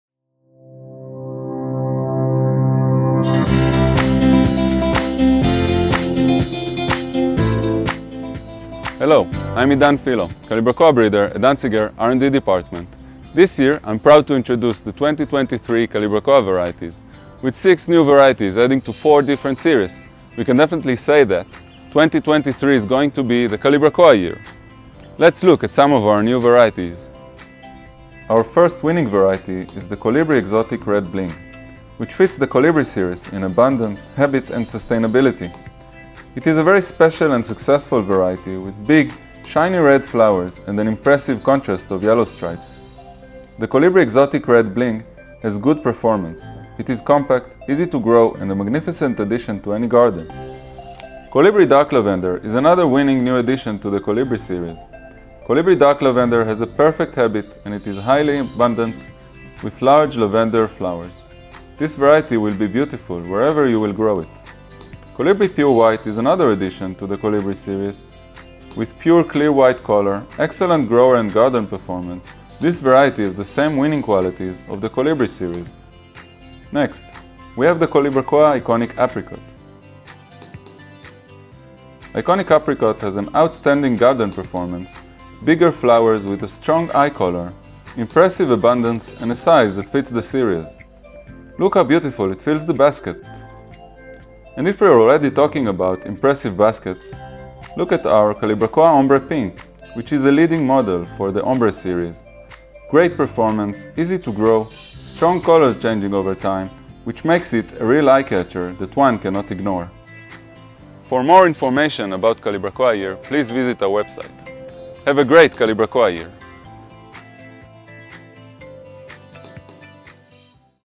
2022 - The year of the Calibrachoa
Idan Filo, Calibrachoa breeder at the Danziger R&D department, introduces 6 Calibrachoa varieties for 2023:
Some of our highlights include:
COLIBRI™ Exotic Red Bling
COLIBRI™ Dark Lavender
COLIBRI™ Pure White
EYECONIC™ Apricot
OMBRE™ Pink

Danziger is a leader in innovative and imaginative floriculture solutions, offering high quality rooted cuttings of annuals, perennials and cut flowers.